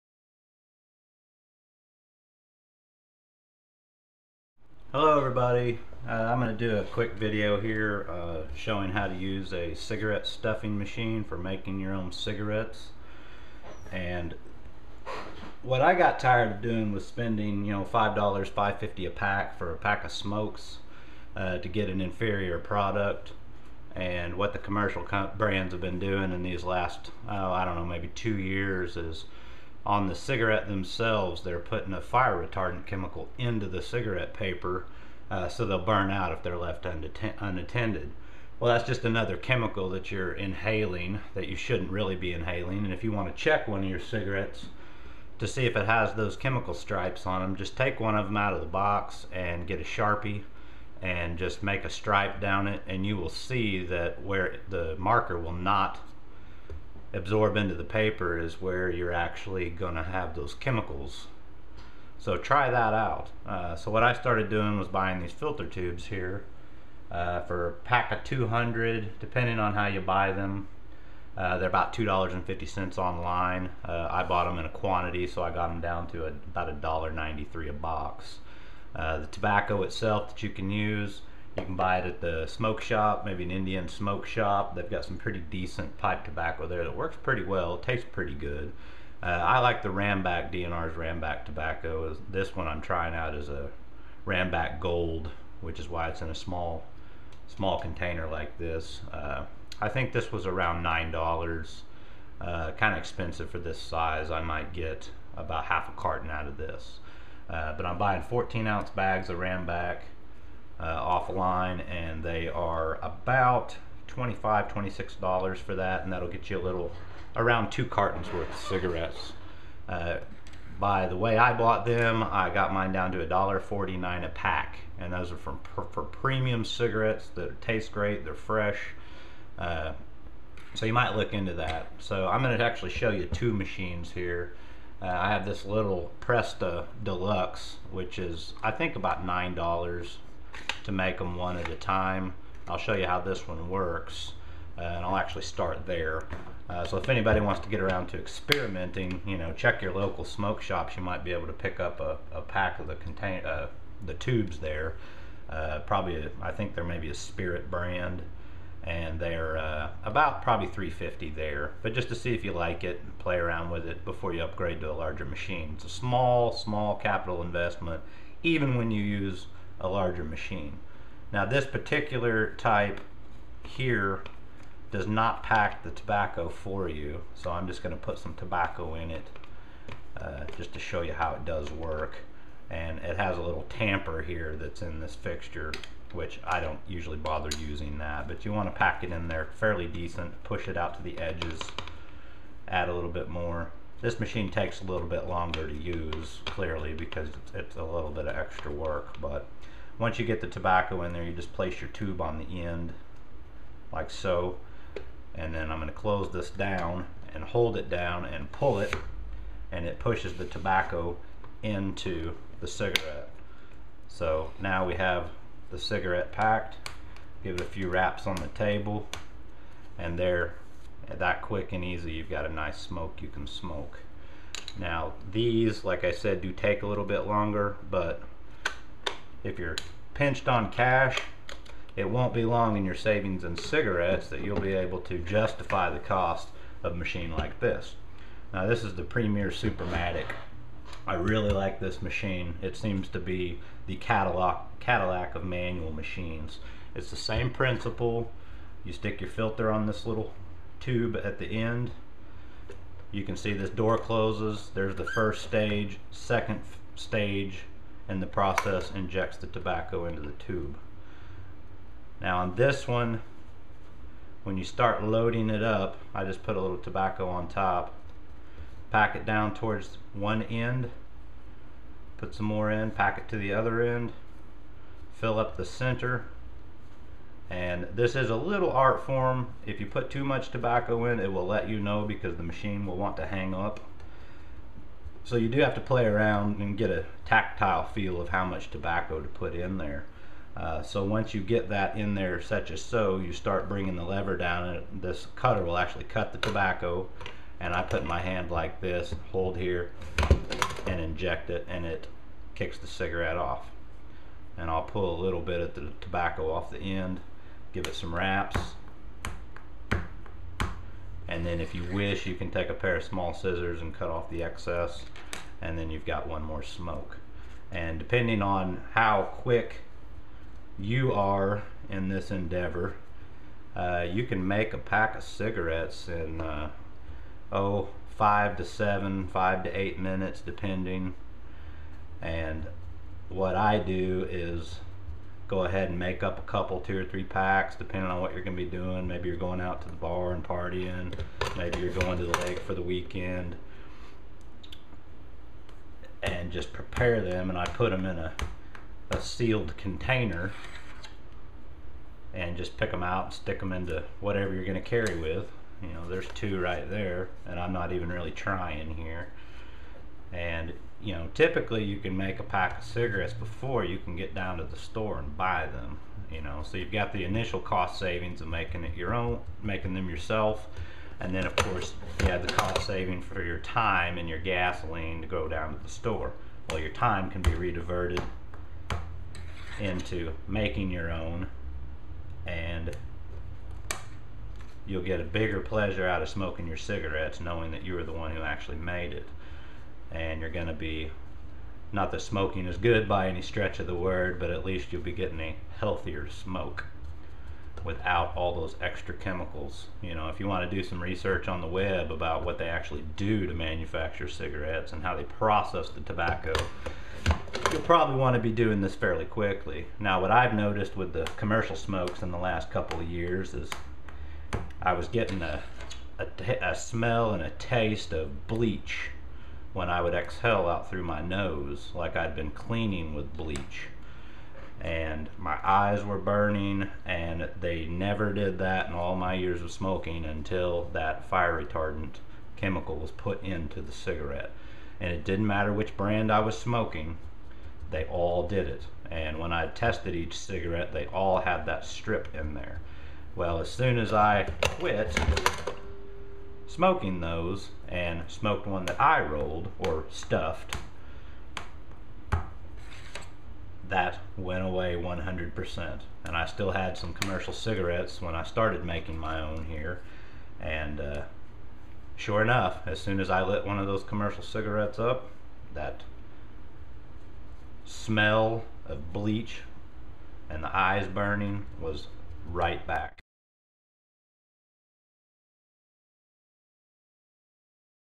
Part 1-2 How to Make Cigarettes with Premier Supermatic.
How to make your own cigarettes with a Premier Supermatic and save money.